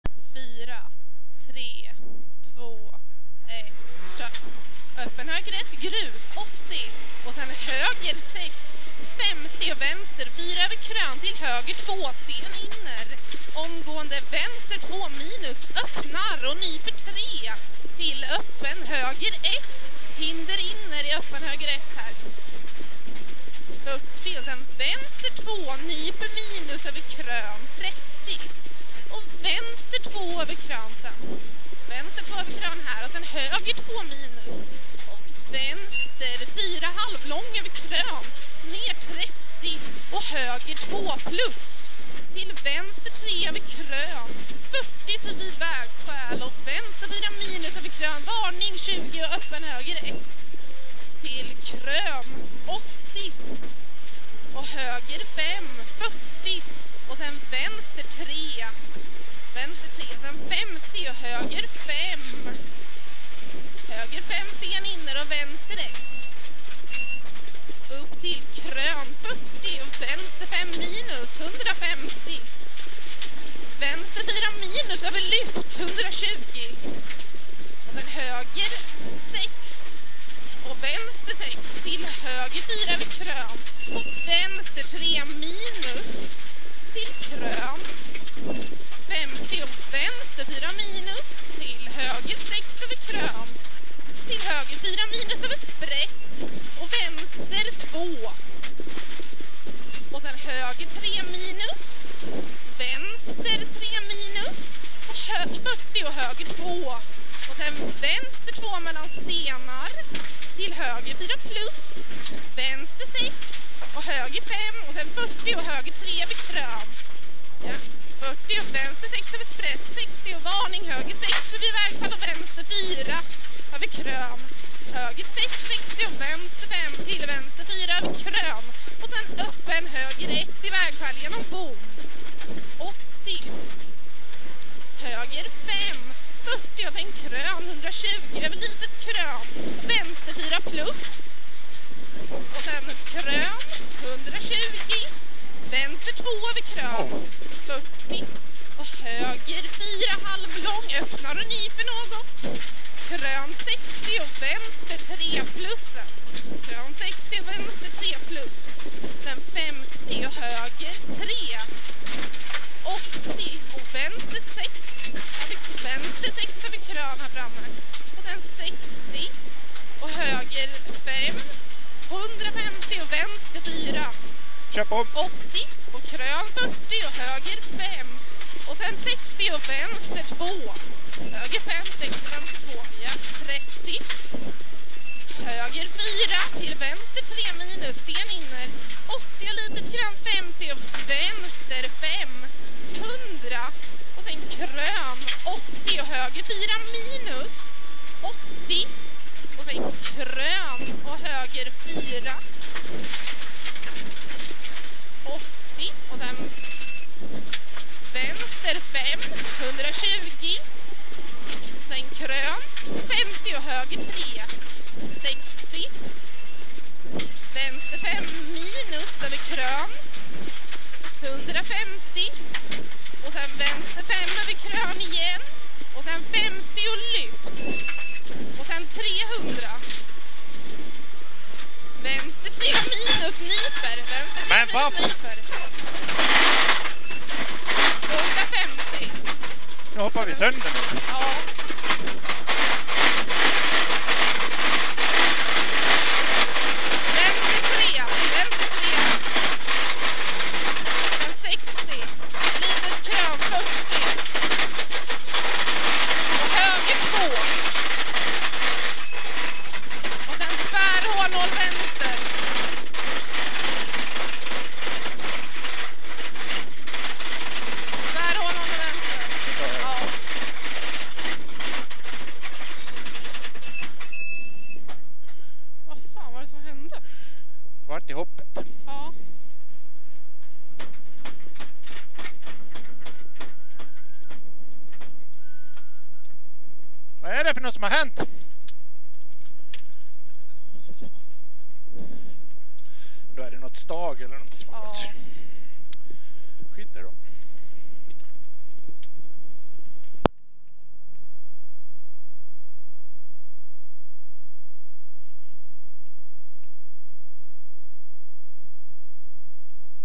Sigtuna 2013 ss 1
Markus Hällbo Motorsport åker Rally Sigtuna ss1.